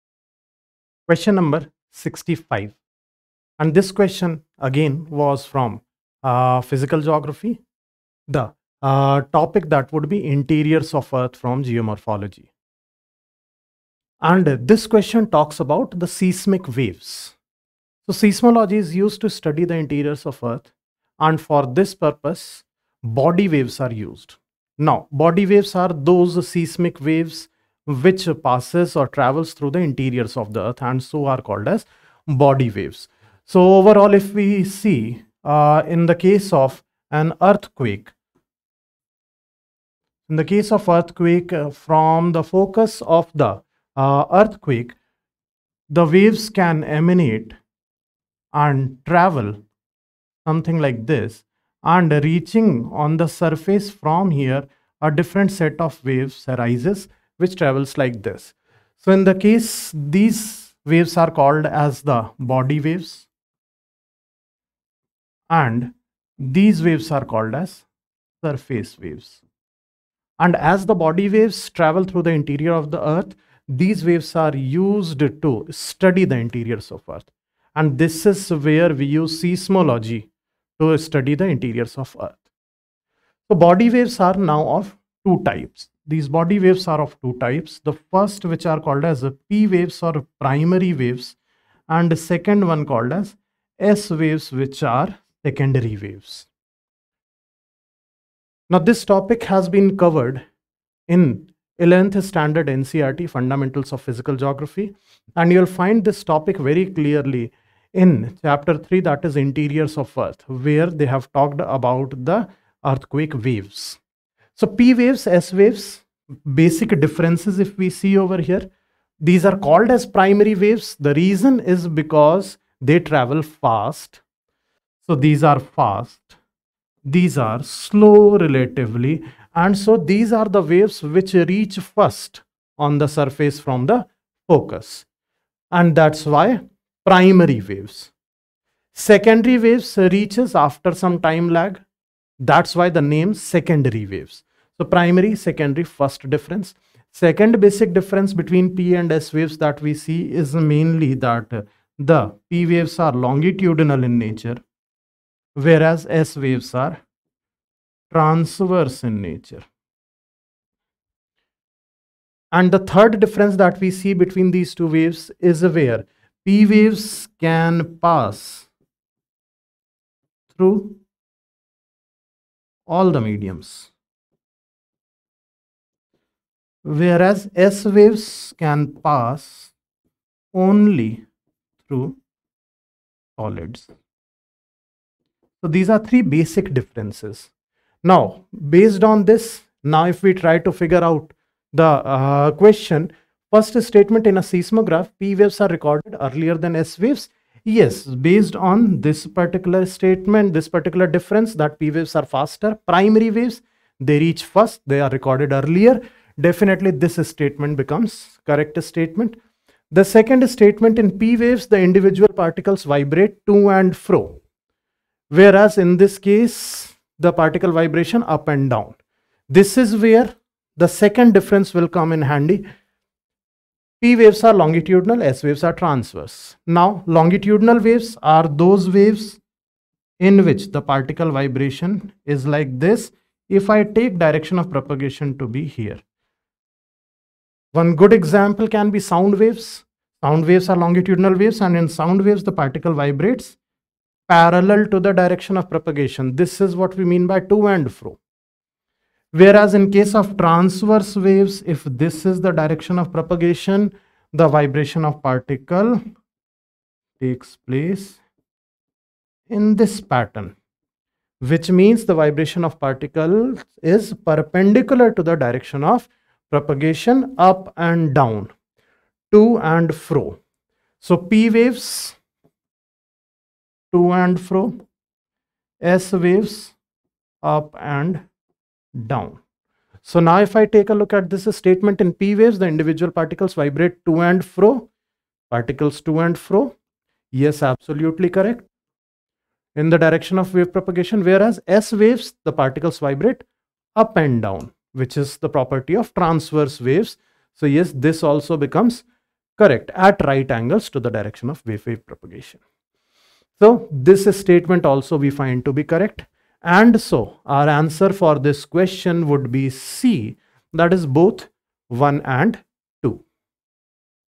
UPSC CSE Prelims 2023 | GS Paper 1 | Question - 65 | Set - A | Answer & Explanation | Rau's IAS
🎯Download UPSC Prelims 2023, UPSC GS Paper I - Analysis, Answer Key & Solutions
🎯Question 65 : Consider the following statements:
1.   In a seismograph, P waves are recorded earlier than S waves.
2.   In P waves, the individual particles vibrate to and fro in the direction of wave propagation whereas in S waves, the particles vibrate up and down at right angles to the direction of wave propagation.
Which of the statements given above is/are correct?
There are two types of body waves. They are called P and S-waves. P-waves move faster and are the first to arrive at the surface. These are also called ‘primary waves’. The P-waves are similar to sound waves. They travel through gaseous, liquid and solid materials.
S-waves arrive at the surface with some time lag. These are called secondary waves. An important fact about S-waves is that they can travel only through solid materials. This characteristic of the S-waves is quite important.
P-waves vibrate parallel to the direction of the wave. The direction of vibrations of S-waves is perpendicular to the wave direction in the vertical plane. Hence, they create troughs and crests in the material through which they pass.
🎯 Search for answers or browse through Rau’s IAS library of the best IAS content.
🎯Connect with Rau’s IAS Study Circle